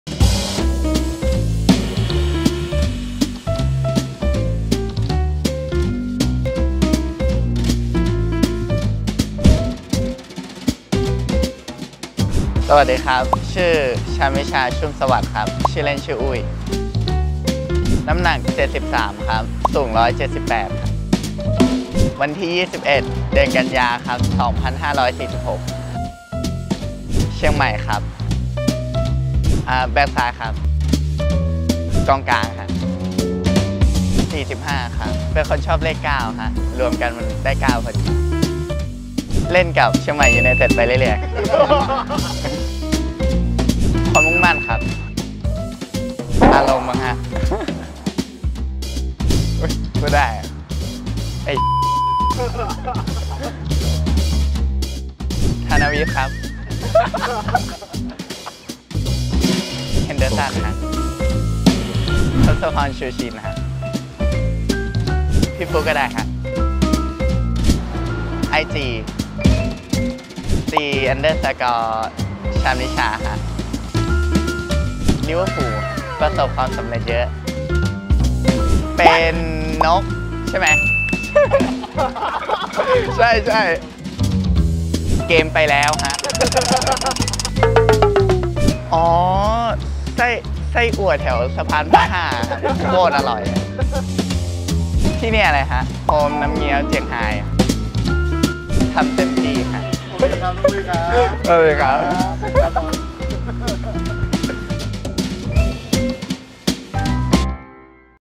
ถามกำ ตอบกำ EP.007 l "อุ้ย" ชาญวิชชา ชุ่มสวัสดิ์
ถามกำตอบกำ EP.007 วันนี้พาแฟนบอลทุกท่านมาพูดคุยแบบสบายๆ ม่วนๆ สไตล์ "ถามกำ ตอบกำ" กับแบคซ้ายดาวรุ่งของทีมอย่าง "อุ้ย" ชาญวิชชา ชุ่มสวัสดิ์
EP.008 จะเป็นใคร หรือคุณอยากให้เราไป "ถามกำ ตอบกำ" กับใครในทีม บอกเราได้เลยนะ รับรองเดี๋ยวจัดให้ !!!
ติดตามทุกการอัพเดตของสโมสรได้ทาง